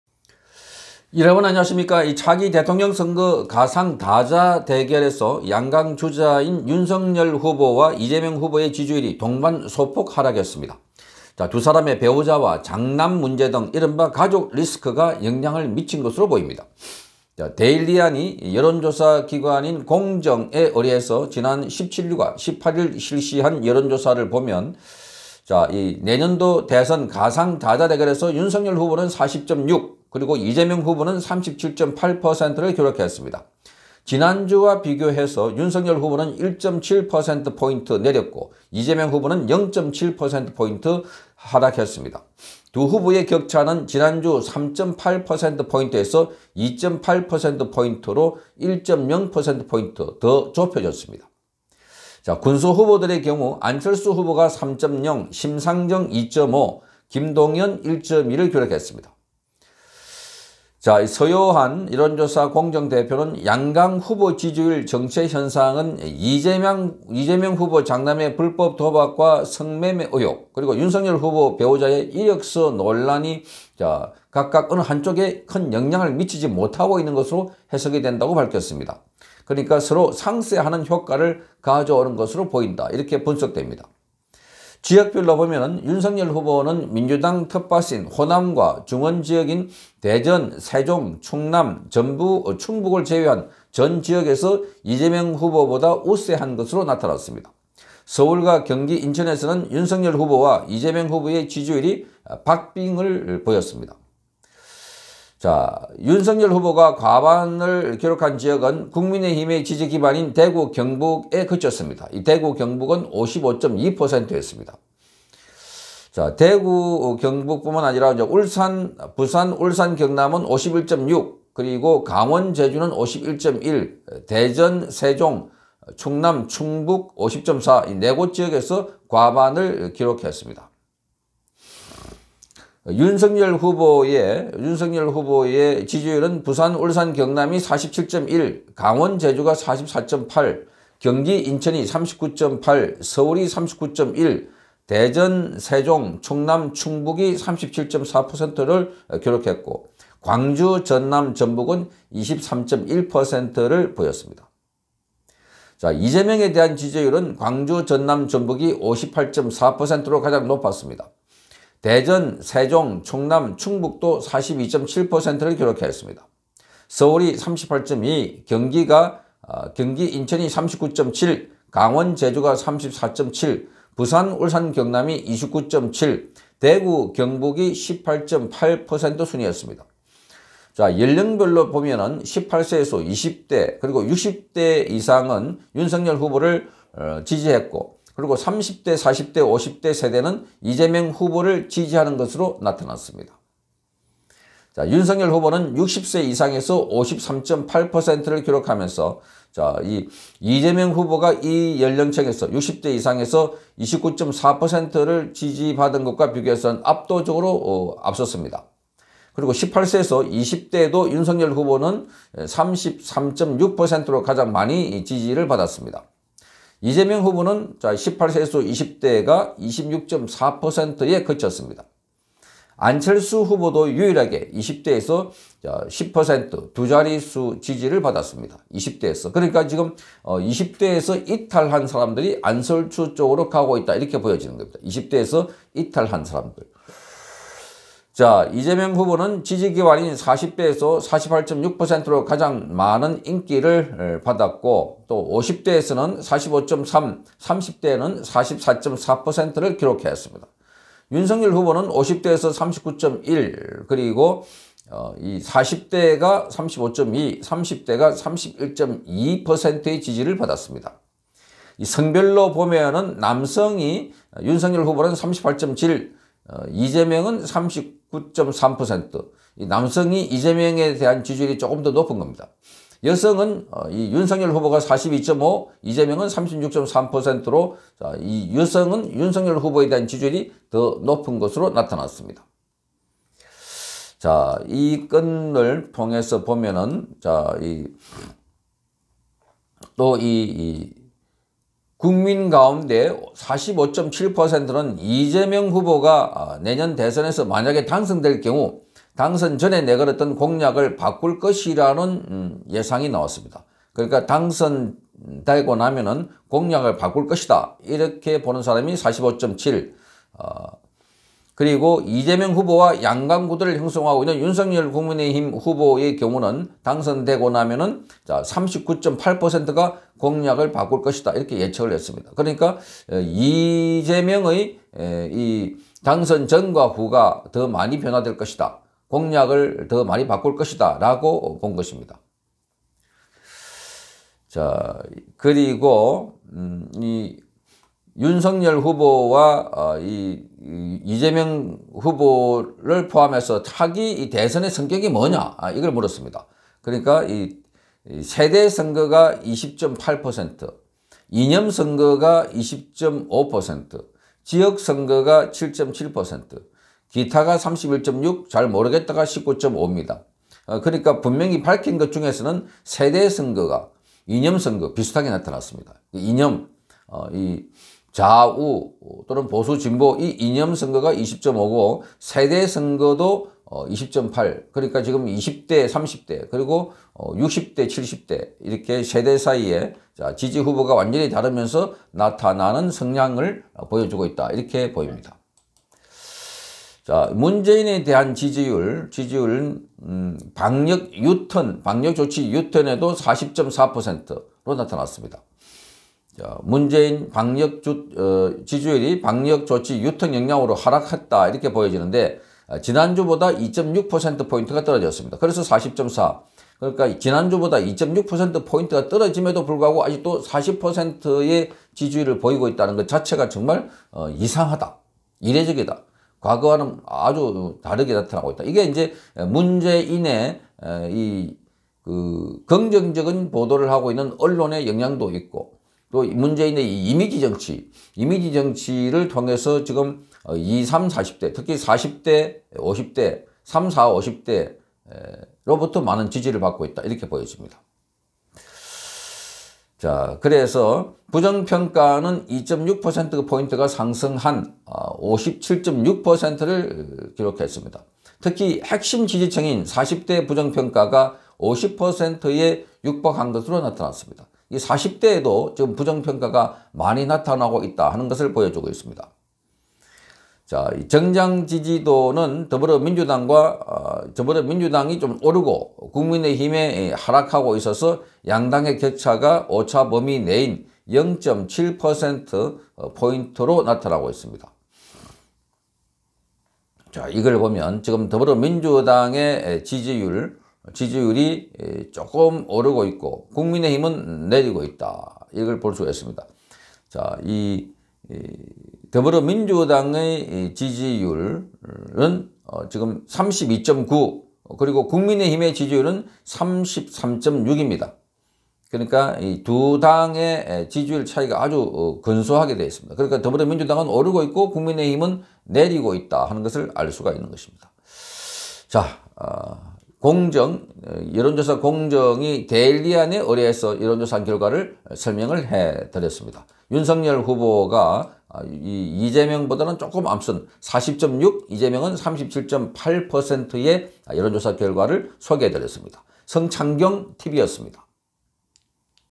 이재명 아들 도박 성매매 의혹과 김건희 이력서 의혹이 어느 한쪽에 타격 주지 못하고 동반하라
윤석열 40.6% 이재명 37.8%...다자대결서 동반 하락.데일리안